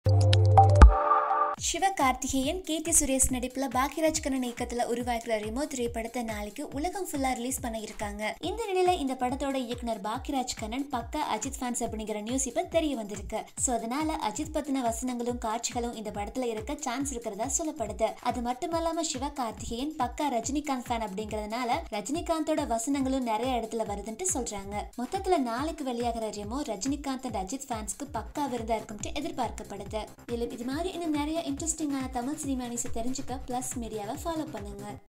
Rajinikanth and Thala Ajith reference in Sivakarthikeyan Remo | Latest Tamil Cinema News | PluzMedia
Rajinikanth and Thala Ajith reference in Sivakarthikeyan Remo. Sivakarthikeyan is a diehard Superstar Rajini fan and Remo director Bhagyaraj Kannan is a Hardcore Thala Fan. It is learnt from the sources that there will be lot Superstar & Thala Reference in Remo. Remo will be a treat for Superstar & Thala Fans.

Sivakarthikeyan, Keerthy Suresh & Sathish starrer Remo is slated for Release this weekend.

For more Kollywood updates and latest Tamil movie news,